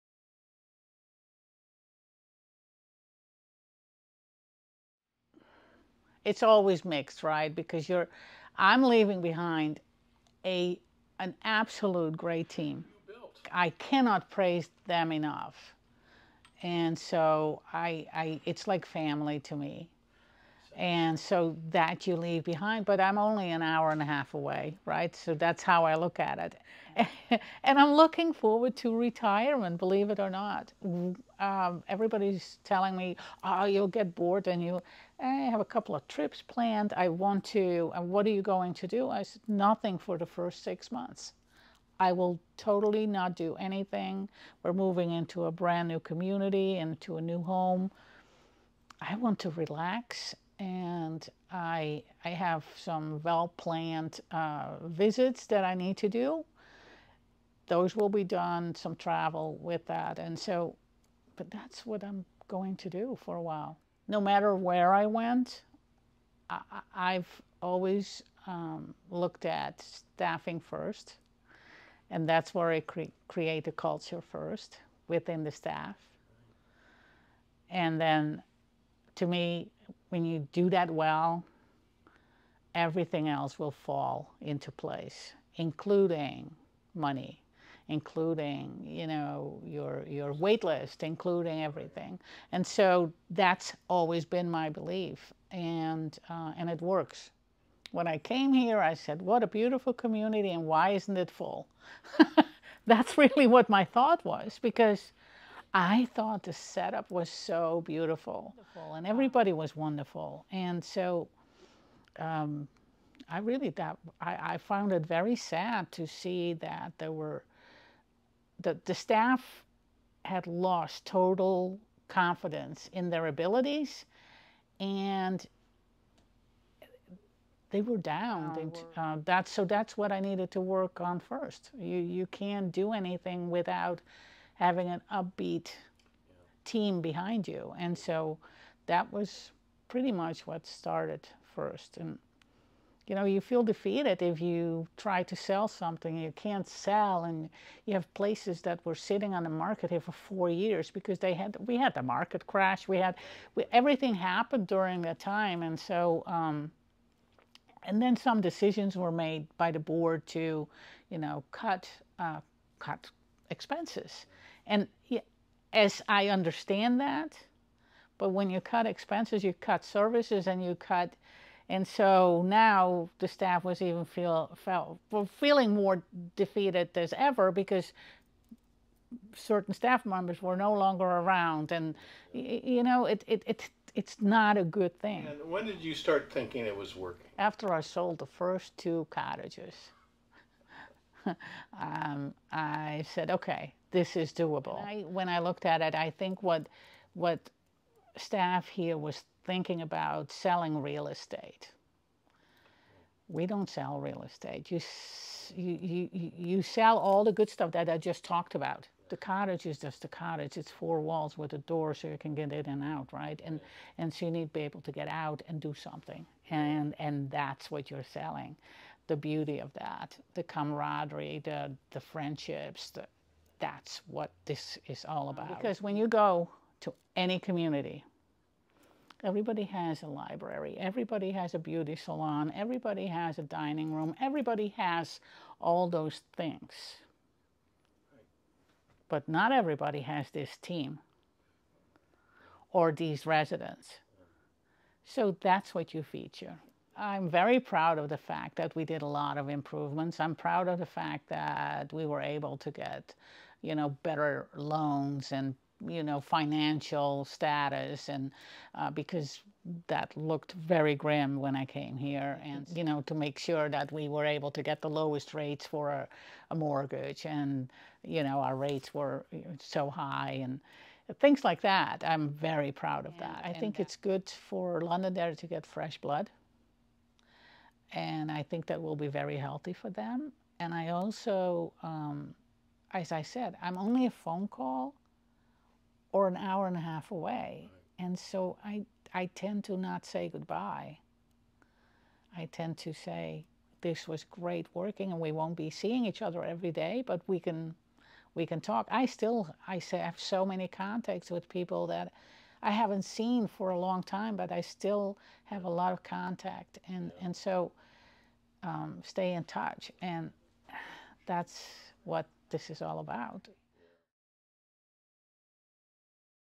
Spy Exit Interview: Londonderry's Irma Toce is Leaving the Building
Having a title like chief executive officer is an appropriate one for the individual tasked with running a multi-million dollar budget as well as overseeing a vast physical campus; Irma Toce never sees her job through the lens of a traditional business leader. Her decades of experience in leading residential retirement communities had told her well before she took on the CEO role at Londonderry at Tred Avon in 2014 that her job was building a unique sense of place there.

For Irma, that rebuilding started with the staff. With morale suffering from the impact of the 2008 recession and high occupancy rates, that sense of place (and purpose) was the missing ingredient. In fact, she still recalls visiting Londonderry during the interview process and was bewildered that such a beautiful place with such wonder residents was having any problems at all.

And that personal mission has been very much in evidence at Londonderry for almost ten years. With full occupancy, healthy financials, and a culture of authentic community, Irma not only helped steady the ship but clearly had a great time doing it.

The Spy continues with our ongoing series of exit interviews with some of the Mid-Shore's most talented individuals as they come to the close of long-term impactful leaders of our leading community organizations and businesses, of which Irma is at the top of the list.